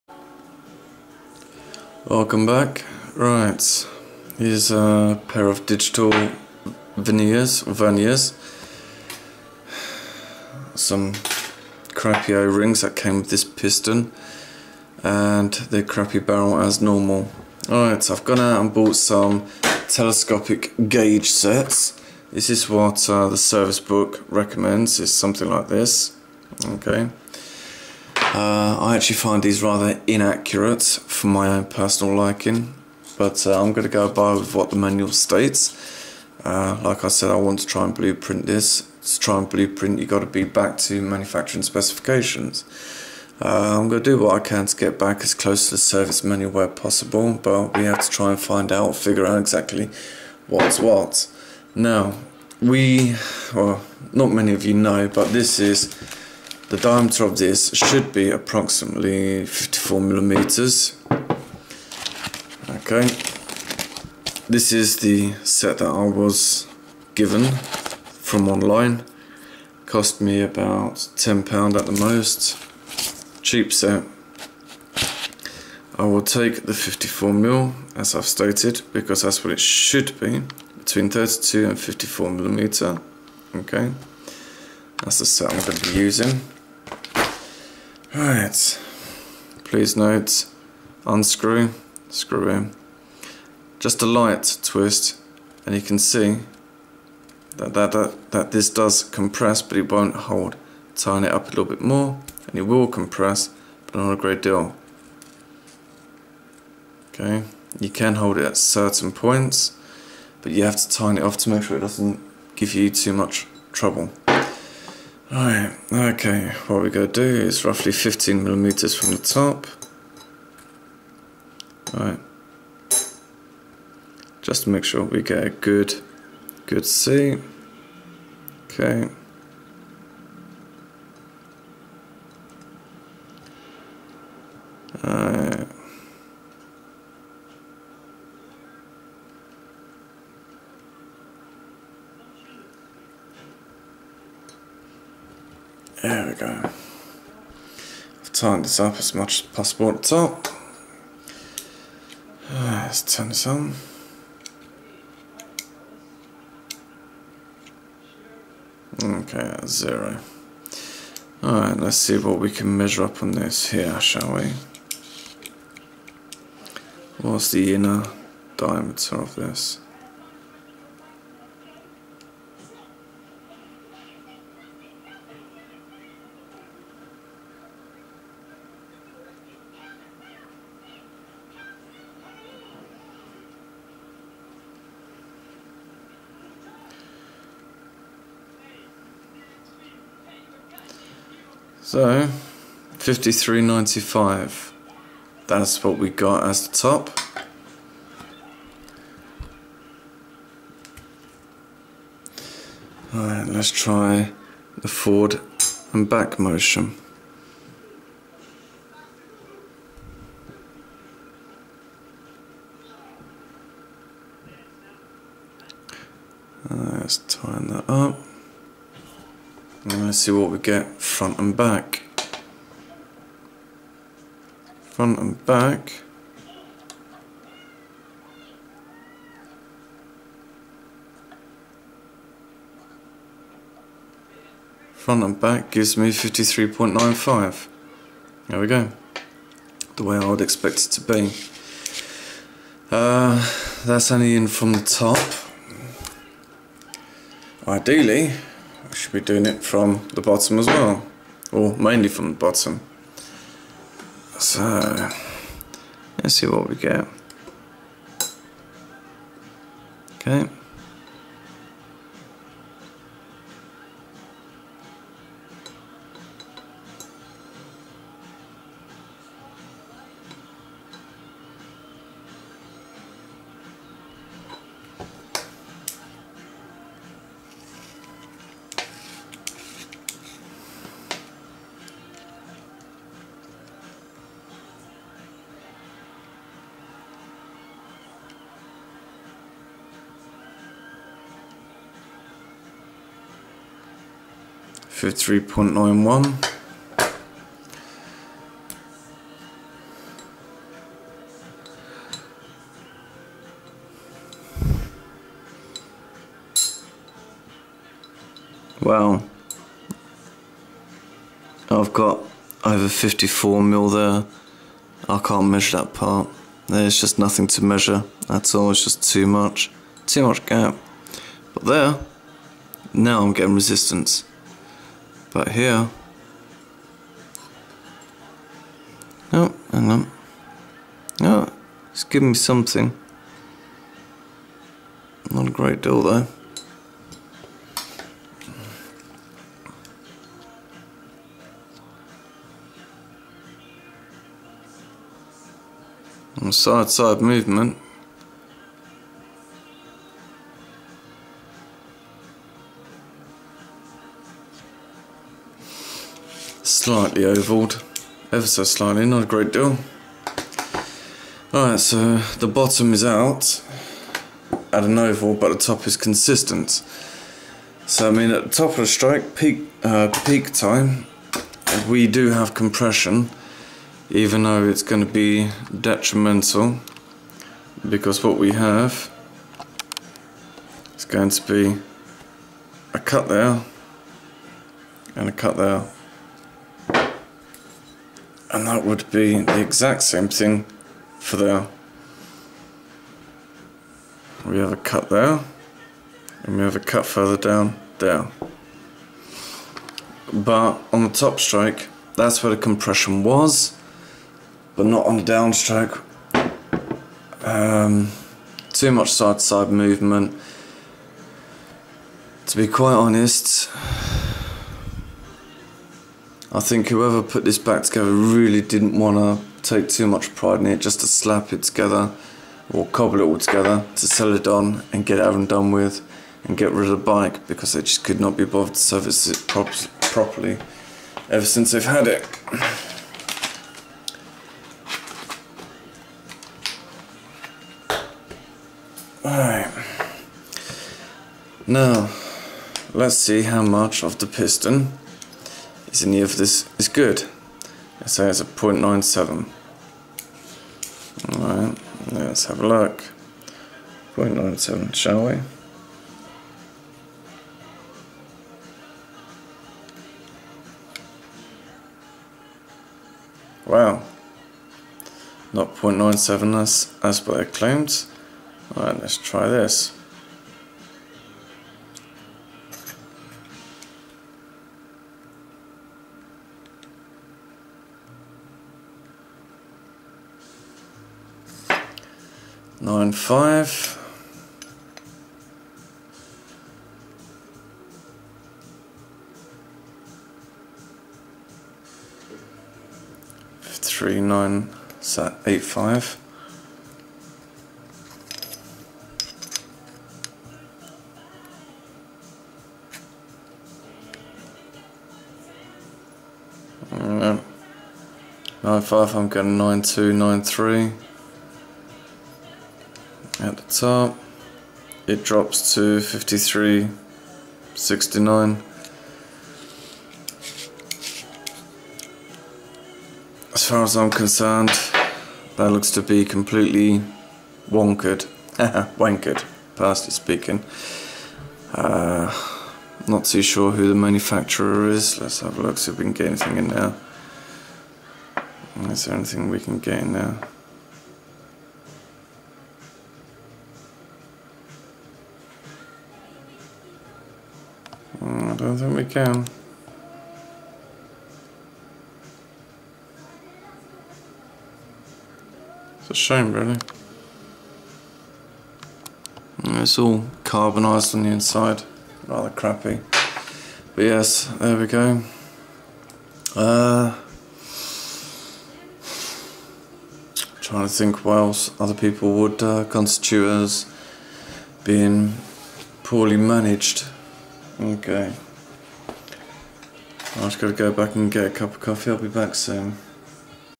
Aprilia RS125 Piston and measurements for BluePrinting before ordering new parts
Aprilia RS125 Piston measurements and repairs.  Measurements taken to satisfy my personal beliefs about the state of the engine.  My personal feeling is that this engine was just slapped together and sold on with little or no regards to the standard of the bike and for the life of the next person.